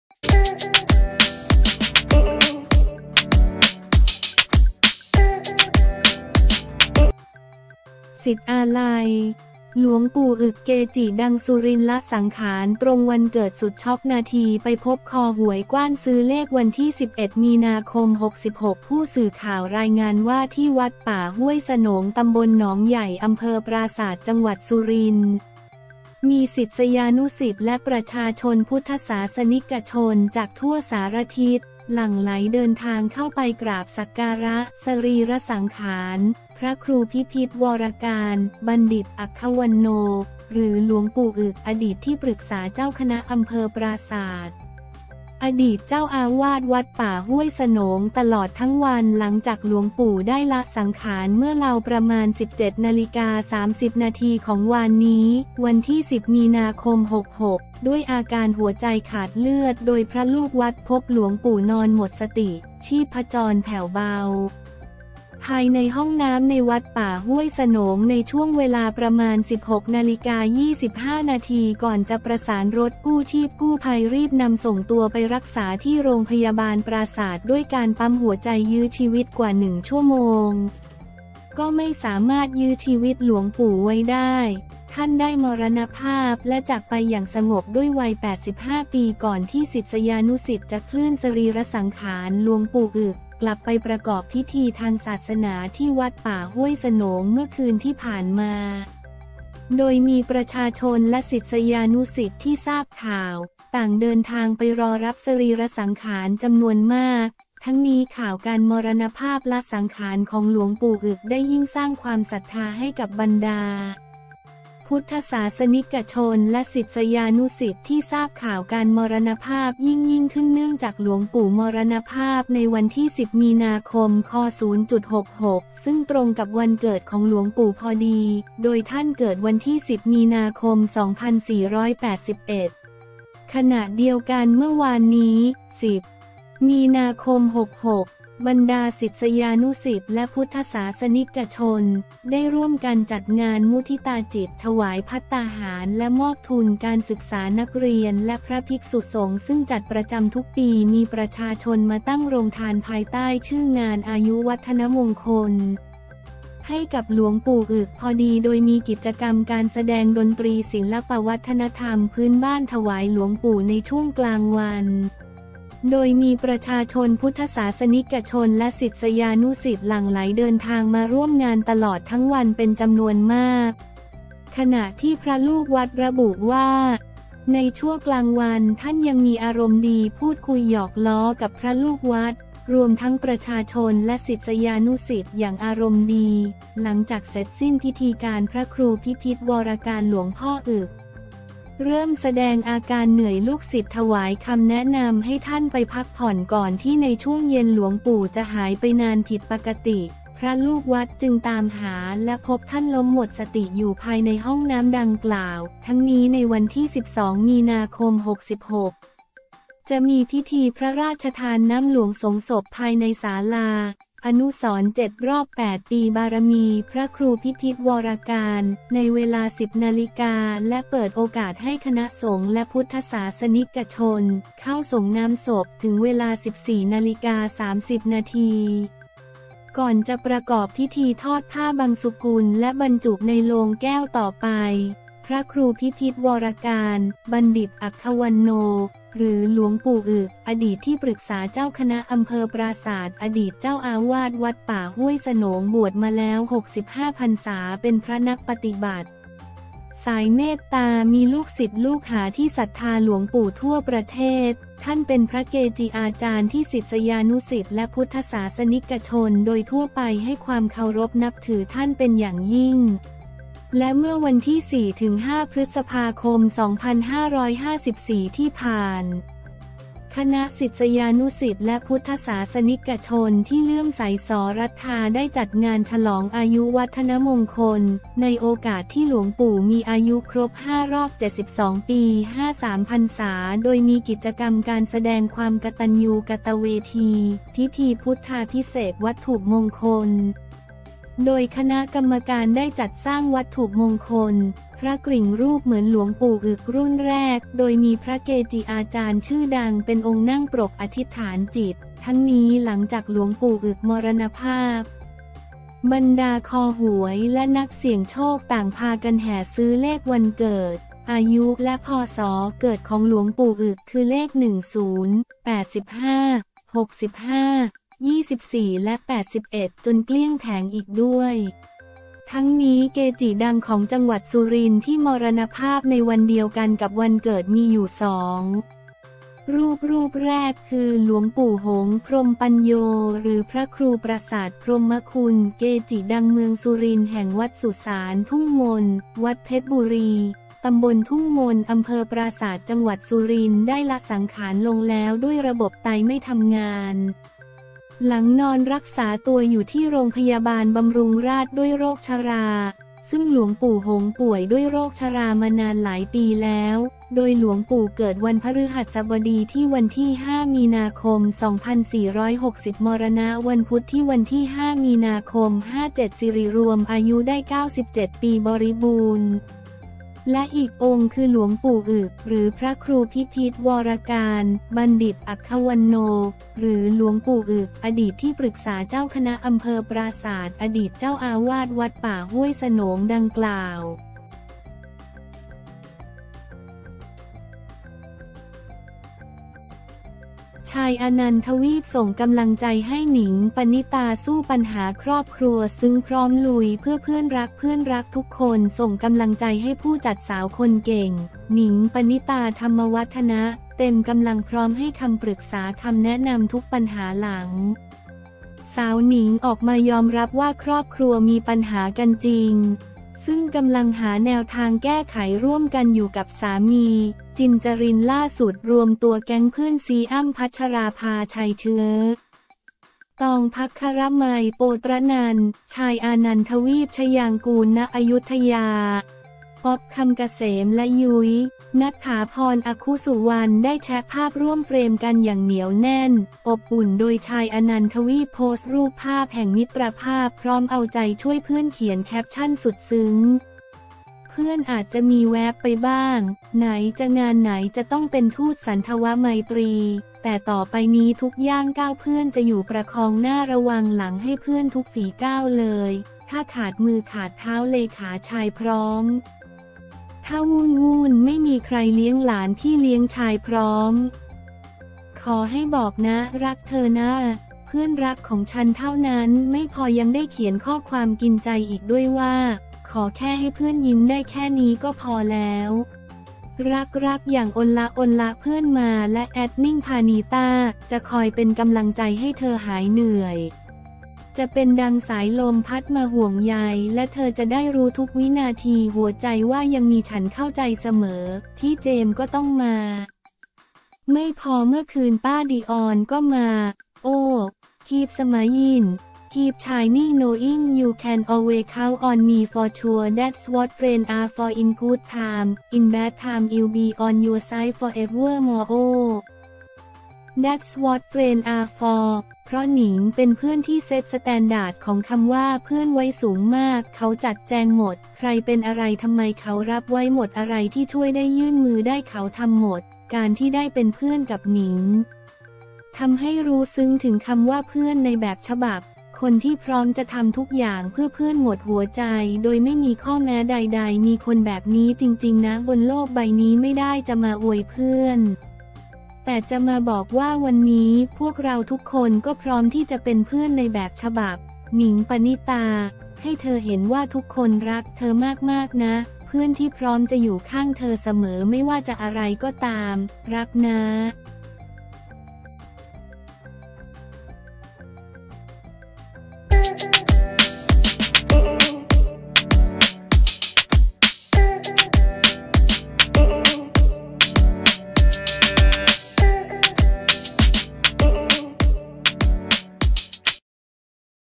ศิษย์อาลัย! หลวงปู่อึก เกจิดังสุรินทร์ ละสังขารตรงวันเกิด สุดช็อกนาทีไปพบ คอหวยกว้านซื้อเลข
วันที่ 11 มี.ค.66 ผู้สื่อข่าวรายงานว่า ที่วัดป่าห้วยเสนง ต.หนองใหญ่ อ.ปราสาท จ.สุรินทร์ มีศิษยานุศิษย์และประชาชนพุทธศาสนิกชนจากทั่วสารทิศ หลั่งไหลเดินทางเข้าไปกราบสักการะสรีระสังขาร พระครูพิพิธวรการ (บัณฑิต อัควัณโณ) หรือหลวงปู่อึก อดีตที่ปรึกษาเจ้าคณะ อ.ปราสาท อดีตเจ้าอาวาสวัดป่าห้วยเสนง ตลอดทั้งวัน
หลังจากหลวงปู่ได้ละสังขาร เมื่อเลาประมาณ 17.30 น.ของวานนี้ (10 มี.ค.66) ด้วยอาการหัวใจขาดเลือด โดยพระลูกวัดพบหลวงปู่นอนหมดสติ ชีพจรแผ่วเบา ภายในห้องน้ำในวัดป่าห้วยเสนง ในช่วงเวลาประมาณ 16.25 น.ก่อนจะประสานรถกู้ชีพกู้ภัยรีบนำส่งตัวไปรักษาที่ รพ.ปราสาท ด้วยการปั๊มหัวใจยื้อชีวิตกว่า 1 ชม. ก็ไม่สามารถยื้อชีวิตหลวงปู่ไว้ได้ ท่านได้มรณภาพและจากไปอย่างสงบด้วยวัย 85 ปี ก่อนที่ศิษยานุศิษย์จะเคลื่อนสรีระสังขารหลวงปู่อึก กลับไปประกอบพิธีทางศาสนาที่วัดป่าห้วยเสนงเมื่อคืนที่ผ่านมา โดยมีประชาชนและศิษยานุศิษย์ที่ทราบข่าว ต่างเดินทางไปรอรับสรีระสังขารจำนวนมาก
ทั้งนี้ข่าวการมรณภาพ ละสังขารของหลวงปู่อึก ได้ยิ่งสร้างความศรัทธาให้กับบรรดา พุทธศาสนิกชนและศิษยานุศิษย์ที่ทราบข่าวการมรณภาพยิ่งๆ ขึ้น เนื่องจากหลวงปู่มรณภาพในวันที่ 10 มี.ค.ค.66 ซึ่งตรงกับวันเกิดของหลวงปู่พอดี โดยท่านเกิดวันที่ 10 มี.ค.2481 ขณะเดียวกัน เมื่อวานนี้ (10 มี.ค.66) บรรดาศิษยานุศิษย์และพุทธศาสนิกชน ได้ร่วมกันจัดงานมุทิตาจิต ถวายภัตตาหาร และ มอบทุนการศึกษานักเรียน และพระภิกษุสงฆ์ ซึ่งจัดประจำทุกปี มีประชาชนมาตั้งโรงทาน ภายใต้ชื่องานอายุวัฒนมงคล ให้กับหลวงปู่อึกพอดี

โดยมีกิจกรรมการแสดงดนตรีศิลปวัฒนธรรมพื้นบ้านถวายหลวงปู่ในช่วงกลางวัน โดยมีประชาชนพุทธศาสนิกชนและศิษยานุศิษย์หลั่งไหลเดินทางมาร่วมงานตลอดทั้งวันเป็นจำนวนมาก ขณะที่พระลูกวัด ระบุว่า ในช่วกลางวันท่านยังมีอารมณ์ดีพูดคุยหยอกล้อกับพระลูกวัด รวมทั้งประชาชนและศิษยานุศิษย์อย่างอารมณ์ดี หลังจากเสร็จสิ้นพิธีการ พระครูพิพิธวรการ หลวงพ่ออึก เริ่มแสดงอาการเหนื่อยลูกศิษย์ถวายคำแนะนำให้ท่านไปพักผ่อนก่อนที่ในช่วงเย็นหลวงปู่จะหายไปนานผิดปกติ พระลูกวัดจึงตามหาและพบท่านล้มหมดสติอยู่ภายในห้องน้ำดังกล่าว
ทั้งนี้ในวันที่ 12 มี.ค.66 จะมีพิธีพระราชทานน้ำหลวงสรงศพ ภายในศาลา “อนุสรณ์ 7 รอบ 8 ปี บารมีพระครูพิพิธวรการ” ในเวลา 10.00 น.และเปิดโอกาสให้คณะสงฆ์และพุทธศาสนิกชน เข้าสรงน้ำศพถึงเวลา 14.30 น. ก่อนจะประกอบพิธีทอดผ้าบังสุกุลและบรรจุในโลงแก้วต่อไป

พระครูพิพิธวรการ (บัณฑิต อัควัณโณ) หรือหลวงปู่อึก อดีตที่ปรึกษาเจ้าคณะ อ.ปราสาท อดีตเจ้าอาวาสวัดป่าห้วยเสนง บวชมาแล้ว 65 พรรษา เป็นพระนักปฏิบัติ สายเมตตา มีลูกศิษย์ลูกหาที่ศรัทธาหลวงปู่ทั่วประเทศ ท่านเป็นพระเกจิอาจารย์ที่ศิษยานุศิษย์และพุทธศาสนิกชนโดยทั่วไปให้ความเคารพนับถือท่านเป็นอย่างยิ่ง
และเมื่อวันที่ 4-5 พฤษภาคม 2554 ที่ผ่าน คณะศิษยานุศิษย์และพุทธศาสนิกชนที่เลื่อมใสศรัทธาได้จัดงานฉลองอายุวัฒนมงคล ในโอกาสที่หลวงปู่มีอายุครบ 5 รอบ 72 ปี 53 พรรษา โดยมีกิจกรรมการแสดงความกตัญญูกตเวที พิธีพุทธาพิเษก วัตถุมงคล โดยคณะกรรมการได้จัดสร้างวัตถุมงคล พระกริ่งรูปเหมือนหลวงปู่อึก รุ่นแรก โดยมีพระเกจิอาจารย์ชื่อดังเป็นองค์นั่งปรกอธิษฐานจิต